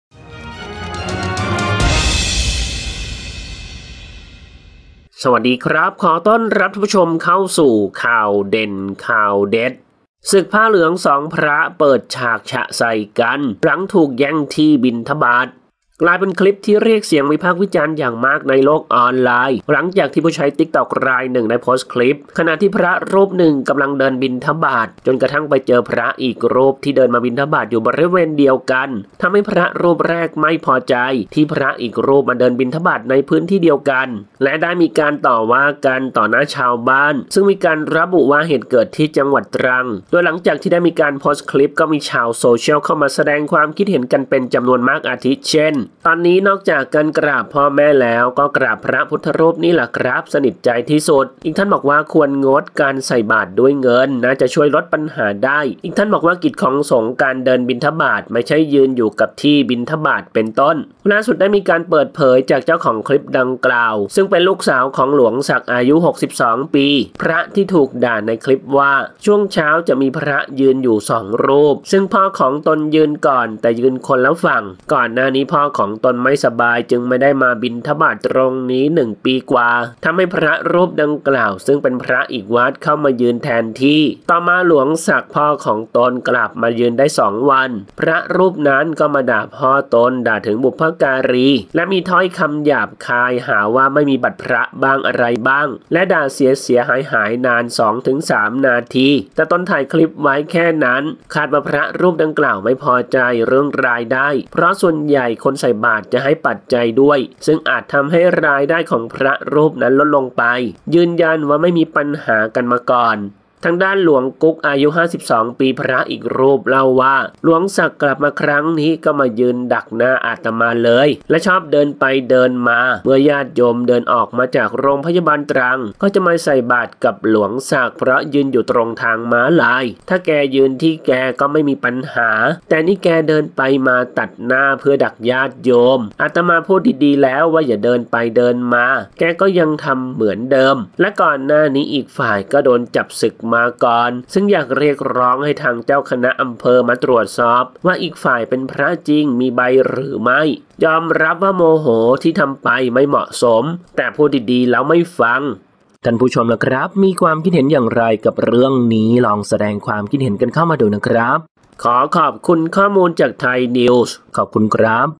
ศึกผ้าเหลือง 2 พระ เปิดฉากฉะใส่กันหลังถูกแย่งที่บิณฑบาต
กลายเป็นคลิปที่ที่เรียกเสียงวิพากษ์วิจารณ์เป็นอย่างมากในโลกออนไลน์ หลังจากที่ผู้ใช้บัญชี tiktok รายหนึ่ง ได้มีการโพสต์คลิปขณะที่พระรูปหนึ่งกำลังเดินบิณฑบาต จนกระทั่งไปเจอพระอีกรูปที่เดินมาบิณฑบาตอยู่บริเวณเดียวกัน ทำให้พระรูปแรกไม่พอใจ ที่พระอีกรูปมาเดินบิณบาตในพื้นที่เดียวกัน และได้มีการต่อว่ากันต่อหน้าชาวบ้าน ซึ่งมีการระบุว่าเหตุเกิดที่ จ.ตรัง

โดยหลังจากที่ได้มีการโพสต์คลิป ก็มีชาวโซเชียลเข้ามาแสดงความคิดเห็นกันเป็นจำนวนมาก อาทิเช่น ตอนนี้นอกจากกราบพ่อแม่แล้ว ก็กราบพระพุธทรุปนี่หละครับสนิทใจที่สุด,ควรงดเว้นการใส่บาตรด้วยเงินน่าจะช่วยลดปัญหาได้,กิจของสงฆ์ การเดินบิณฑบาต ไม่ใช่ยืนอยู่กับที่บิณฑบาต

ล่าสุดได้มีการเปิดเผยจากเจ้าของคลิปดังกล่าว ซึ่งเป็นลูกสาวของ หลวงศักดิ์ อายุ 62 ปี พระที่ถูกด่าในคลิปว่า ช่วงเช้าจะมีพระยืนอยู่ 2 รูป ซึ่งพ่อของตนยืนก่อน แต่ยืนคนละฝั่ง ก่อนหน้านี้พ่อของตนไม่สบาย จึงไม่ได้มาบิณฑบาตตรงนี้มา 1 ปีกว่า ทำให้พระรูปดังกล่าว ซึ่งเป็นพระอีกวัด เข้ามายืนแทนที่

ต่อมาเมื่อ หลวงศักดิ์ พ่อของตนกลับมายืนได้ 2 วัน พระรูปนั้นก็มาด่าพ่อตน ด่าถึงบุพการี และมีถ้อยคำหยาบคาย หาว่าไม่มีบัตรพระบ้าง อะไรบ้าง และด่าเสีย ๆ หาย ๆ นาน 2-3 นาที แต่ตนถ่ายคลิปไว้แค่นั้น คาดว่าพระรูปดังกล่าวไม่พอใจเรื่องรายได้ เพราะส่วนใหญ่คนมาใส่บาตรจะให้ปัจจัยด้วย จึงอาจทำให้รายได้ของพระรูปนั้นลดลงไป ยืนยันว่าไม่เคยมีปัญหากันมาก่อน

ทางด้าน หลวงกุ๊ก อายุ 52 ปี พระอีกรูปเล่าว่า หลวงศักดิ์กลับมาครั้งนี้ ก็มายืนดักหน้าอาตมาเลย และชอบเดินไปเดินมา เมื่อญาติโยมเดินออกมาจาก รพ.ตรัง ก็จะมาใส่บาตรกับหลวงศักดิ์ เพราะยืนอยู่ตรงทางม้าลาย ถ้าแกยืนที่แกก็ไม่มีปัญหา แต่นี่แกเดินไปมา ตัดหน้า เพื่อดักญาติโยม อาตมาพูดดี ๆ แล้วว่าอย่าเดินไปเดินมา แกก็ยังทำเหมือนเดิม และก่อนหน้านี้อีกฝ่ายก็โดนจับสึกมาก่อน จึงอยากเรียกร้องให้ทางเจ้าคณะอำเภอมาตรวจสอบด้วยว่าอีกฝ่ายเป็นพระจริง มีใบหรือไม่ ยอมรับว่าโมโห ที่ทำไปไม่เหมาะสม แต่พูดดี ๆ แล้วไม่ฟัง

00:00 - Intro
00:10 - News
01:00 - Minute